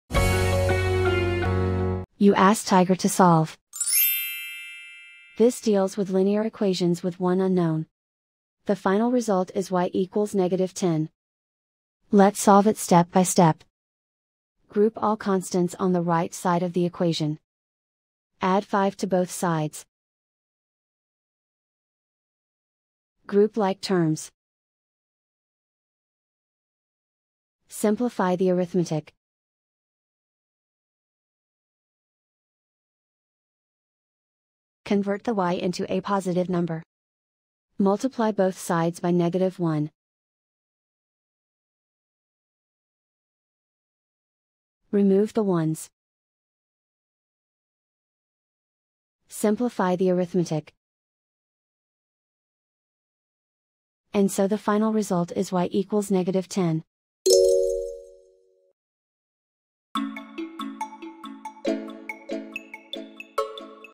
Solve -5-y=5: Linear Equation Video Solution | Tiger Algebra
Struggling with -5-y=5? Watch our step-by-step video on Tiger Algebra to master this linear equation effortlessly.